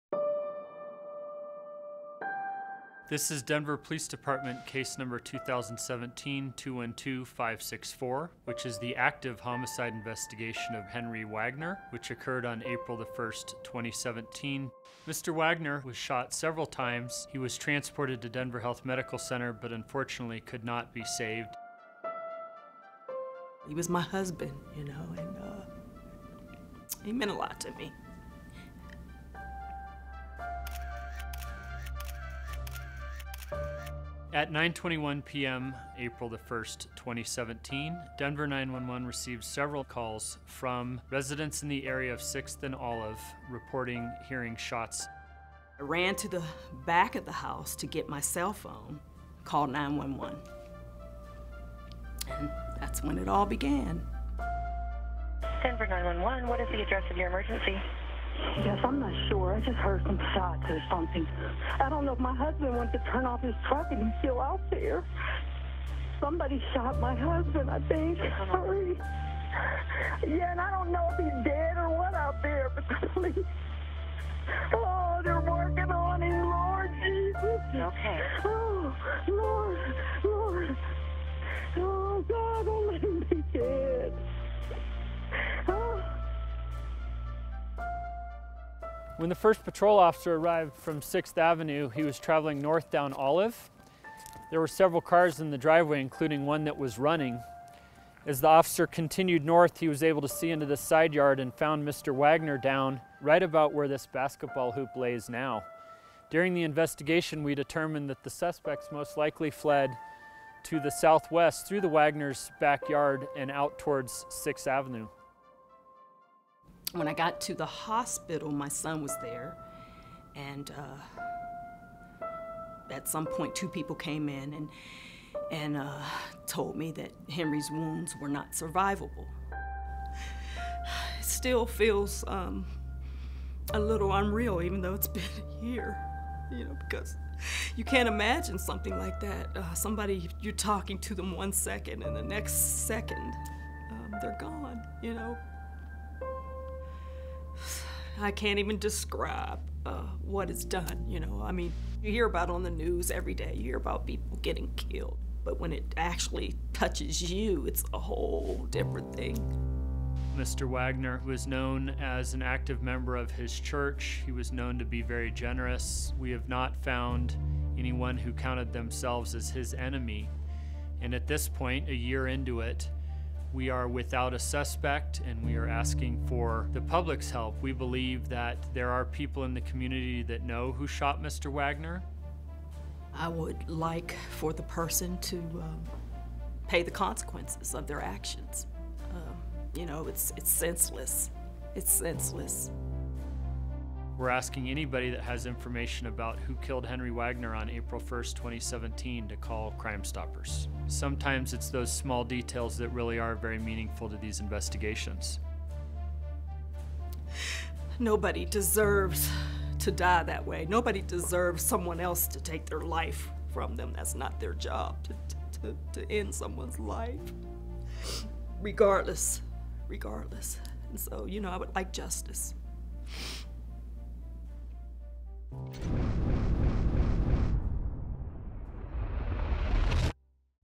COLD CASE: Henry Wagner
Henry Wagner was murdered on April 1, 2017 in Denver's Montclair neighborhood. If you have information about this crime, you can report it anonymously.

For more information on other unsolved homicides in Denver, please